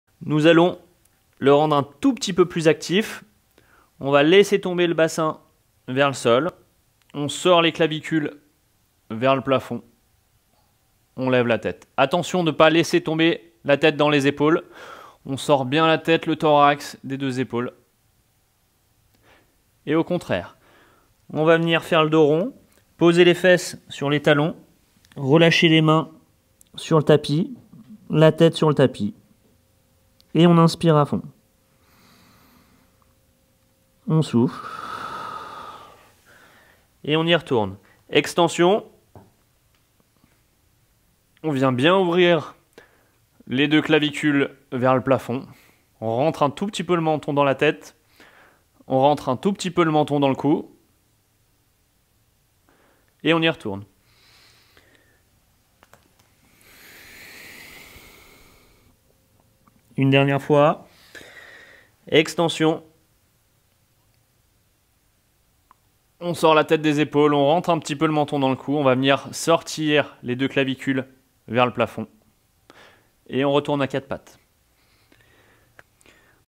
flexion - extension dynamique
flexion extension dynamique pour soulager les pression des disques et ramener de la mobilisation global.